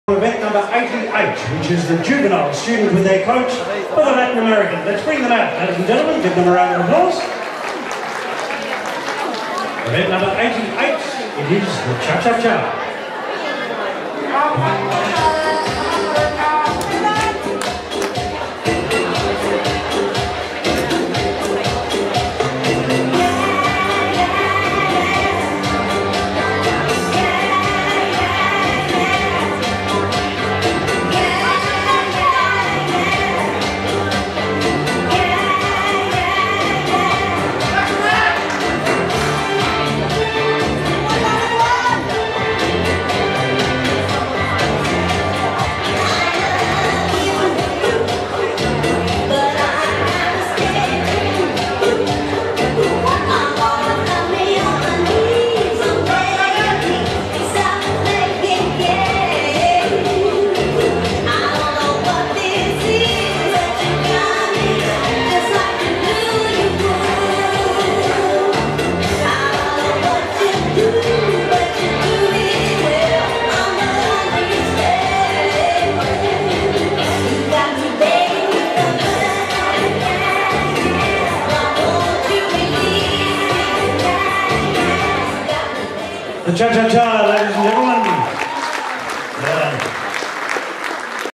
88 CoachStudent Juvenile Latin Cha Final
2024 Australian Dancing Society
Night of Stars
National Championship & Ball
Held on the 15th of June 2024